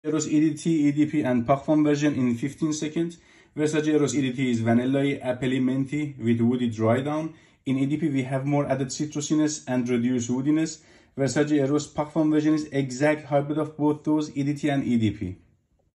Versace Eros edt vs edp and Parfum Comparison | Sexiest Mens cologne fragrance perfume review
A fast comparison on edt, edp and parfum versions of Versace Eros !